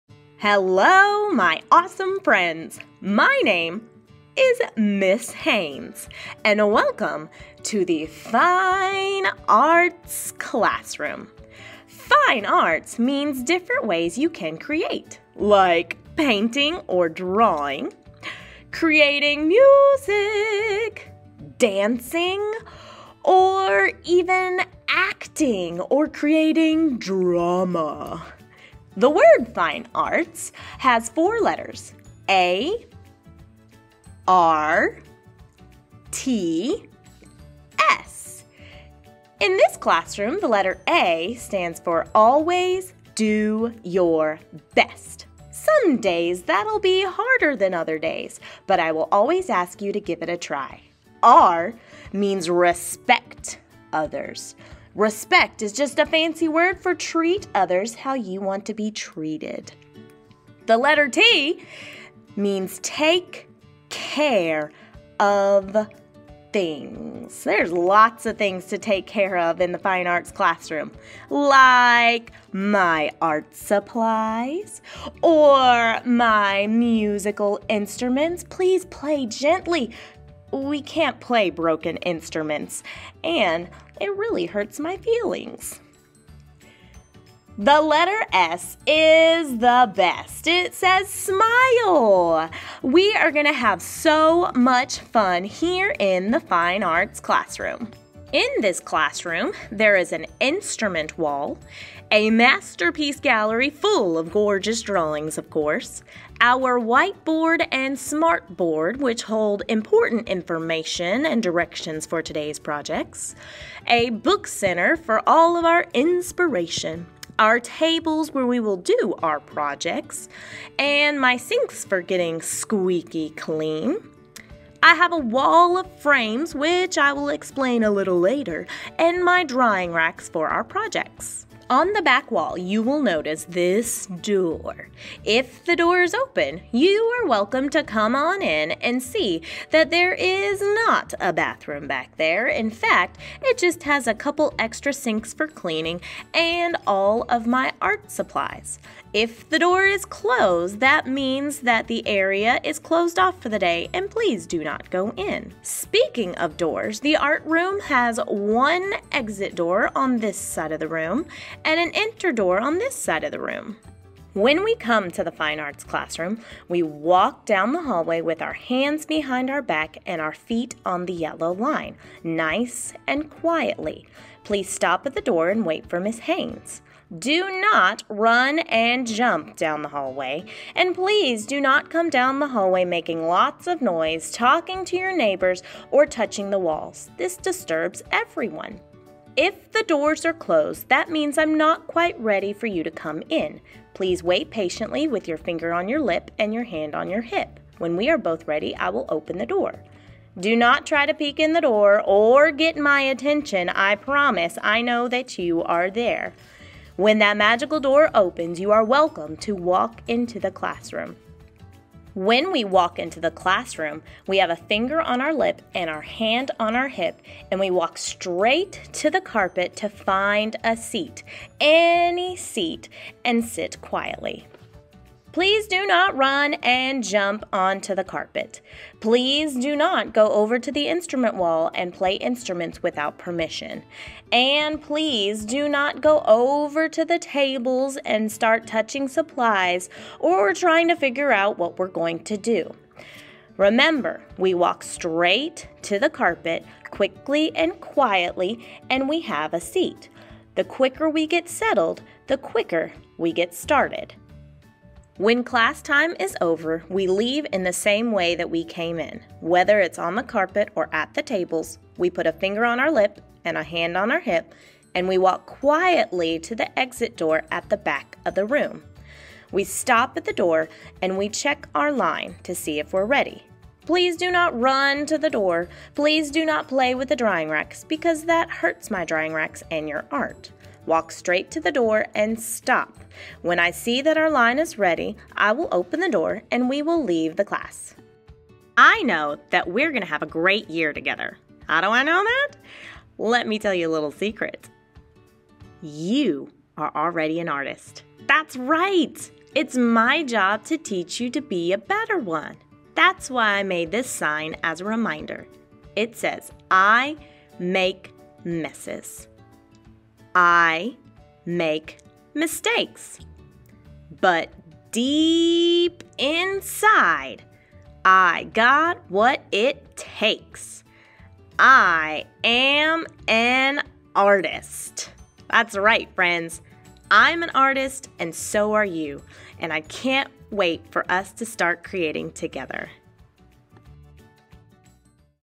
First Day in the Fine Arts Room!
Here is a look at our first day "Welcome" and procedures in Mrs. Haynes' Fine Arts Room.